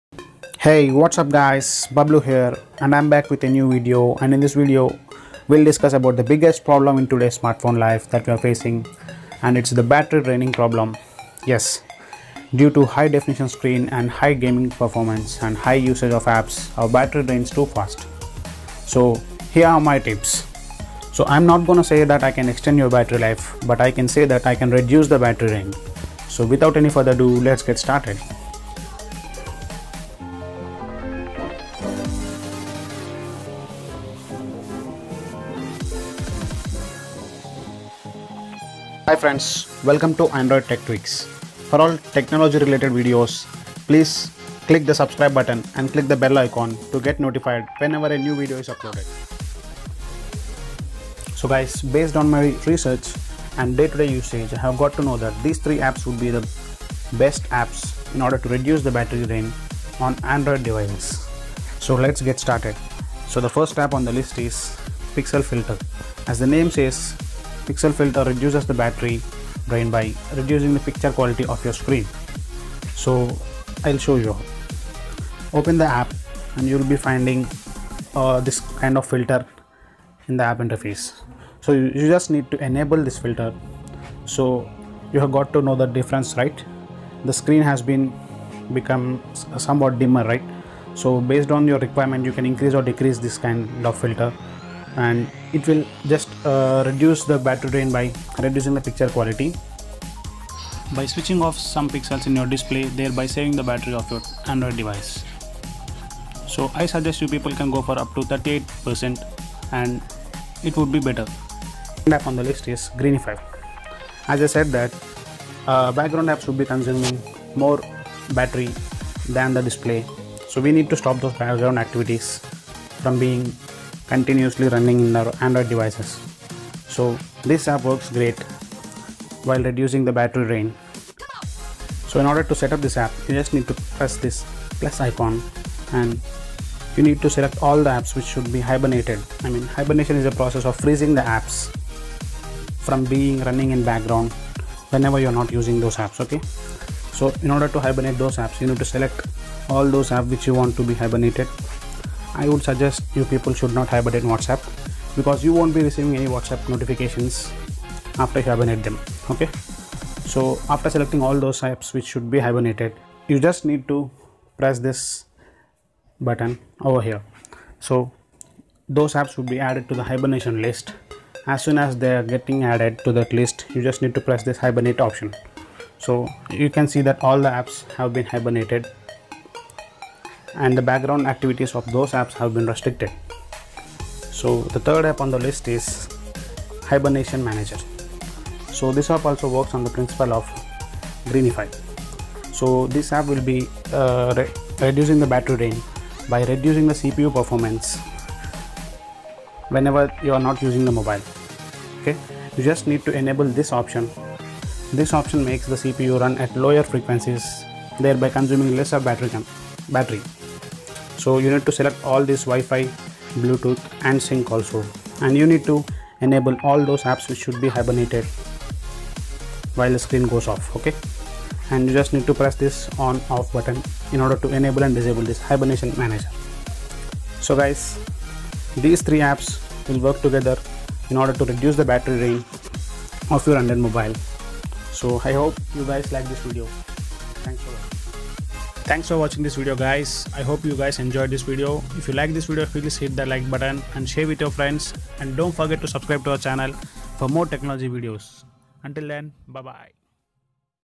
How to increase android battery life - no root
Hi guys in this video we'll discuss about how to reduce battery drain in especially Android mobiles so watch this video till the end and share with your friends and don't forget to subscribe to our channel